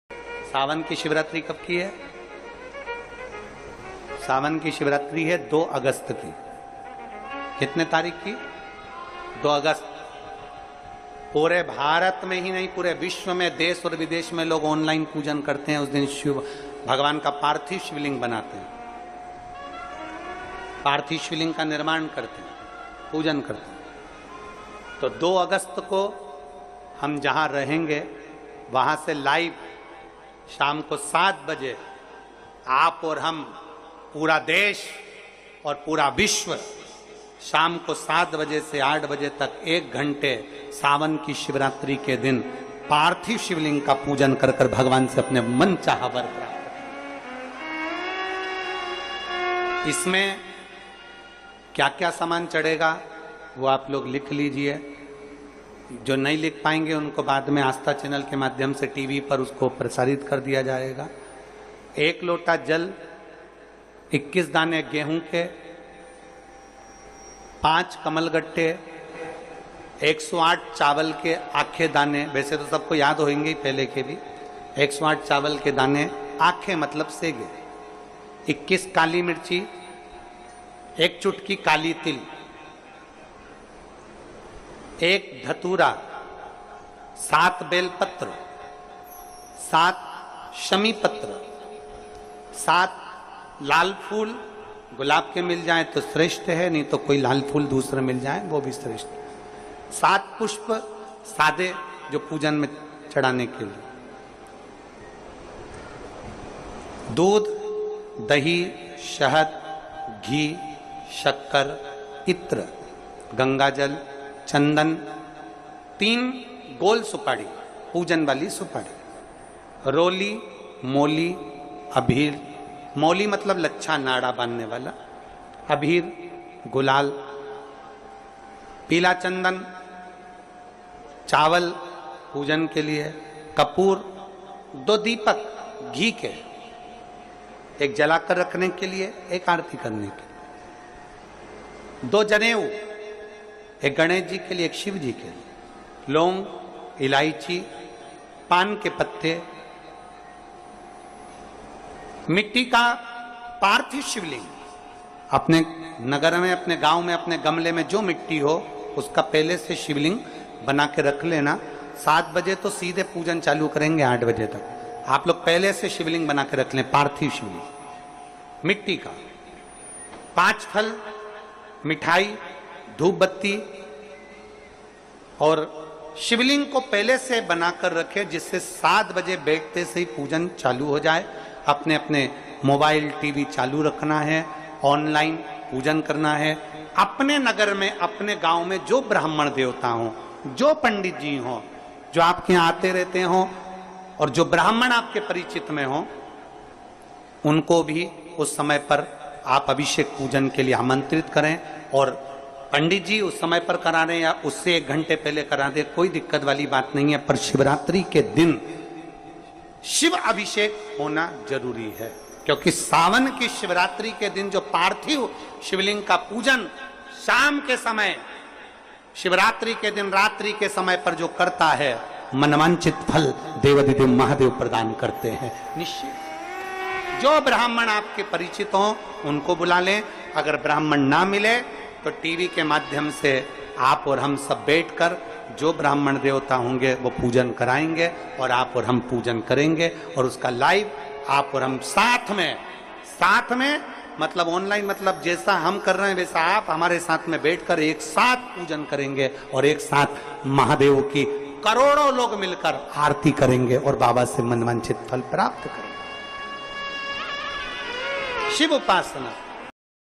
2 अगस्त सावन शिवरात्रि पार्थिव शिवलिंग पूजा सामग्री online लिस्ट शाम 7:00 पूजा शुरू l pradeep mishra
2 अगस्त सावन शिवरात्रि पार्थिव शिवलिंग पूजा सामग्री online लिस्ट शाम 7:00 पूजा शुरू l pradeep mishra